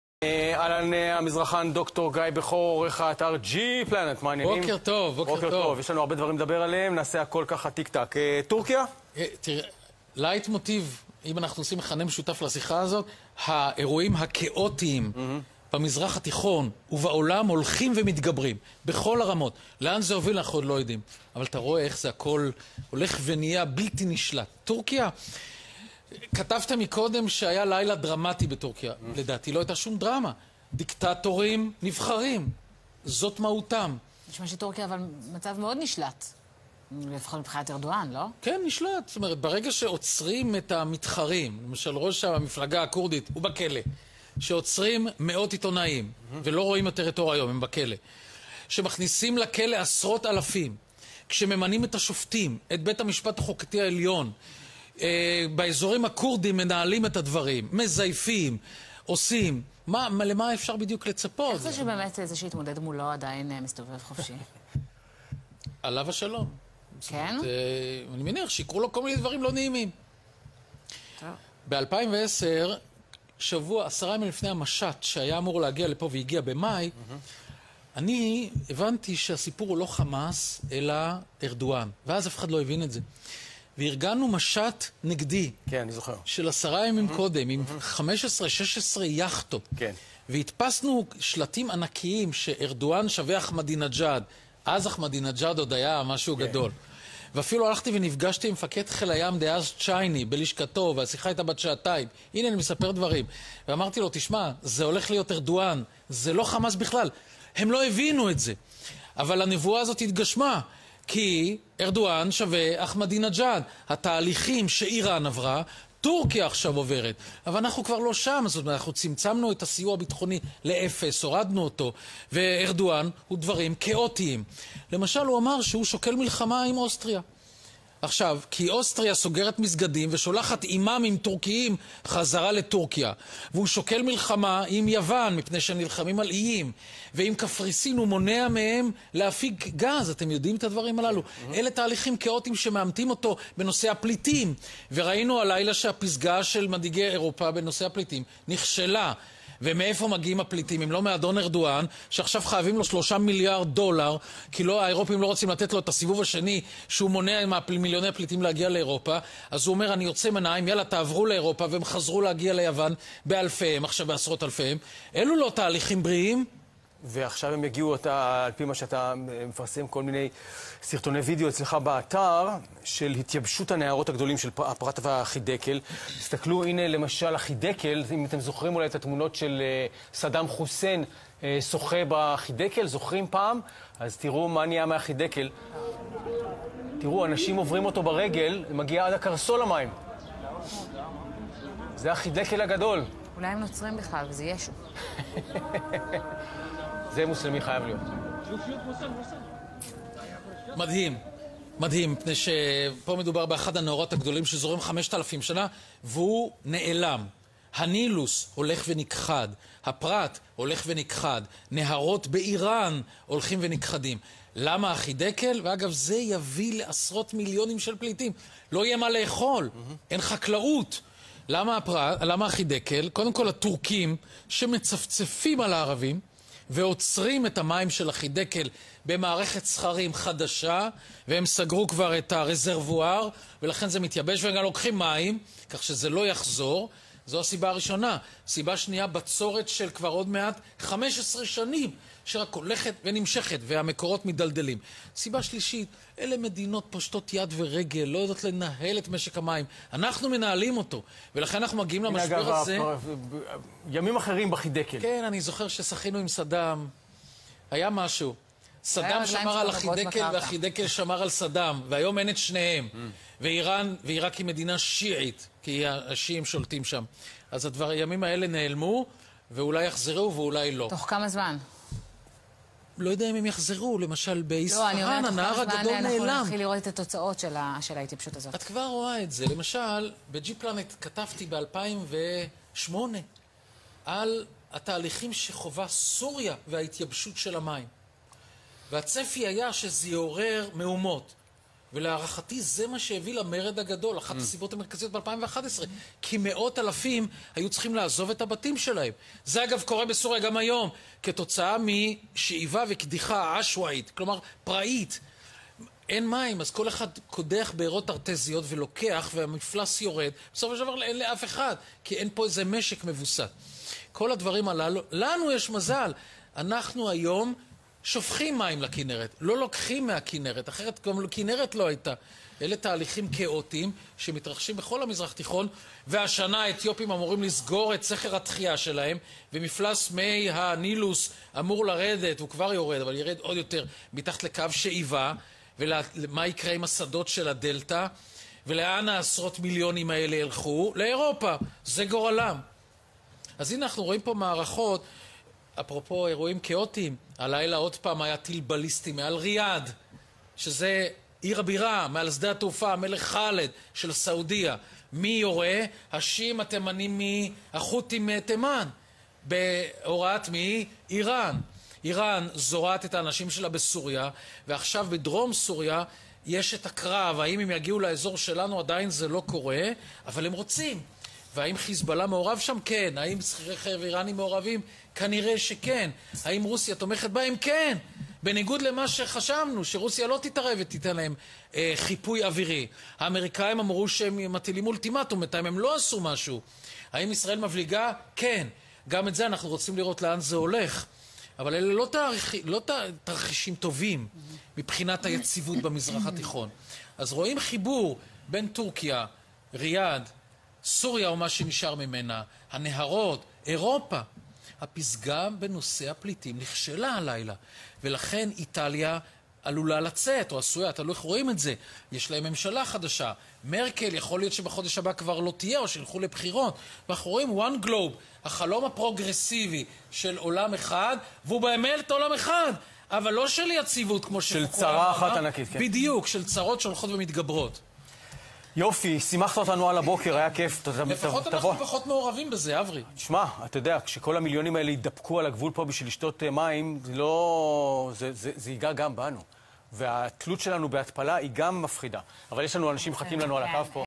גיא בכור מציג: הסיבה העיקרית לתהפוכות במזרח התיכון
ניהול משק מים חובבני ונפשע בצירוף הטיית מים מטעם ממשלת ארדואן.